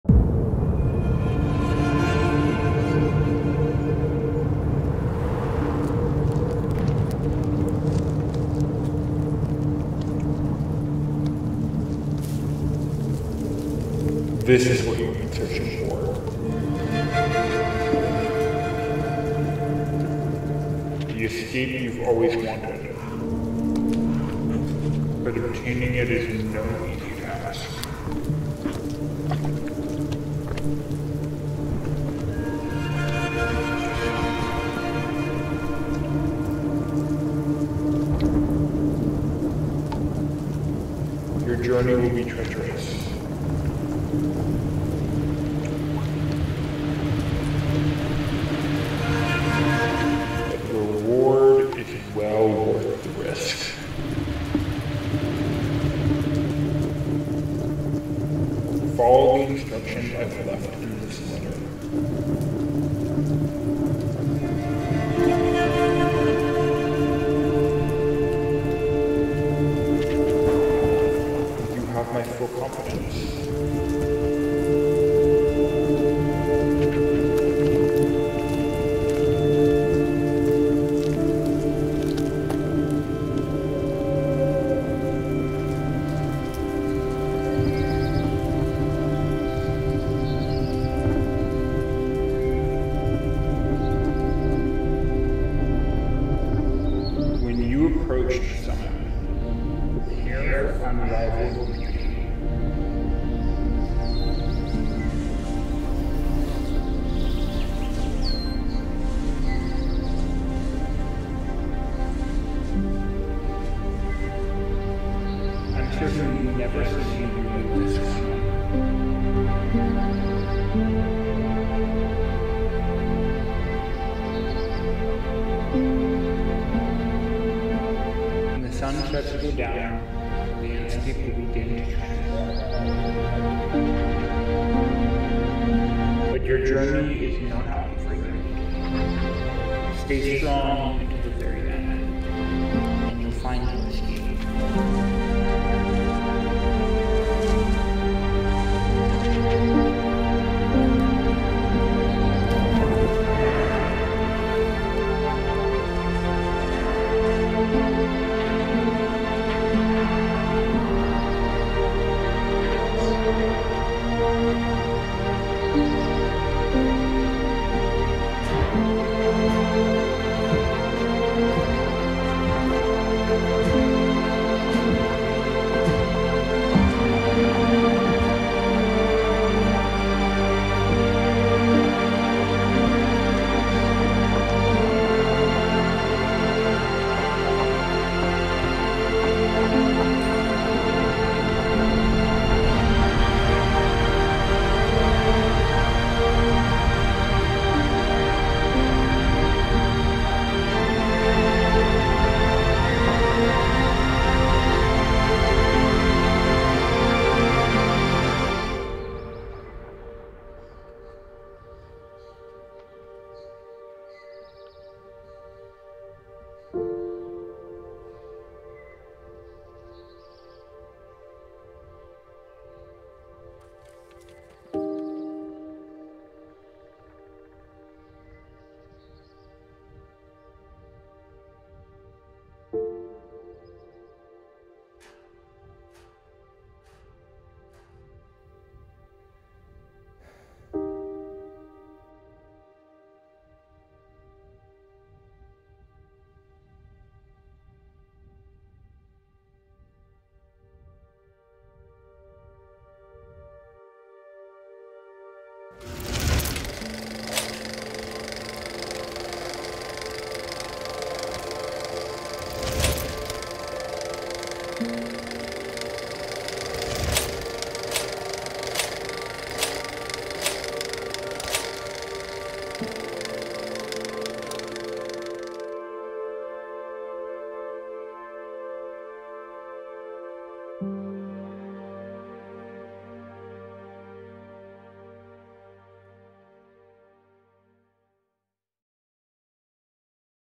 ESCAPE | SHORT FILM | SHOT ON THE CANON EOS R6
ESCAPE follows the journey of The Seeker, who is Seeking an escape from the troubles of his life. He is guided by his friend, The Wanderer, who has wrote the instructions in a letter to The Seeker, so that he may find his Escape.

Actors/Narrators:
The Seeker - Played by Tristan
The Wanderer - Voiced by Nicholas Kaplan

This film was planned, shot, directed, and edited by Rocco Germani

This film was shot almost exclusively on the Canon EOS R6

A special thanks goes to Dustin Gamache for letting us film on his property!